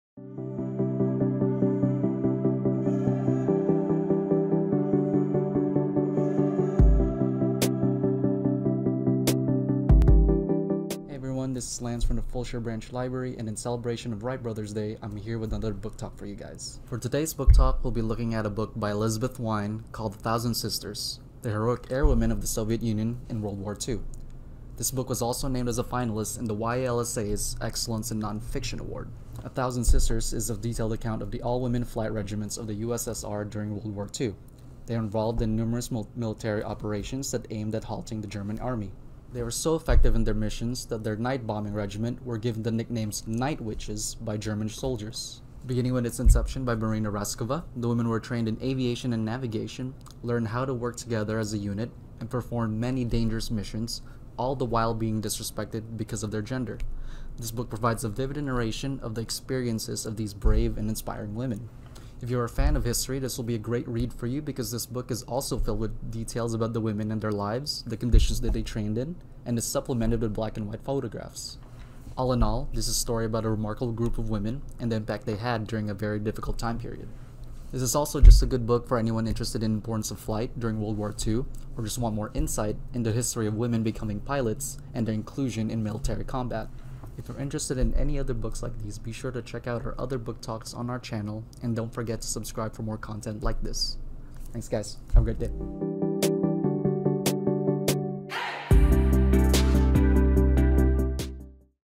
Book Talk: A Thousand Sisters
In FBCL’s Book Talk series, library staff give a synopsis of a featured book and share their thoughts on it. Hear about books that might not have been on your radar – you may discover a new favorite! In this episode, hear about "A Thousand Sisters: The Heroic Airwomen of the Soviet Union in World War II," written by Elizabeth Wein.

In the early years of World War II, Josef Stalin issued an order that made the Soviet Union the first country in the world to allow female pilots to fly in combat. Led by Marina Raskova, three regiments, including the 588th Night Bomber Regiment -- nicknamed the "night witches" -- faced intense pressure and obstacles both in the sky and on the ground. Some of these young women perished in flames. Many of them were in their teens when they went to war.

This is the story of Raskova's three regiments, women who enlisted and were deployed on the front lines of battle as navigators, pilots, and mechanics. It is the story of a thousand young women who wanted to take flight to defend their country, and the woman who brought them together in the sky.

Packed with black-and-white photographs, fascinating sidebars, and thoroughly researched details, "A Thousand Sisters" is the inspiring true story of a group of women who set out to change the world, and the sisterhood they formed even amid the destruction of war.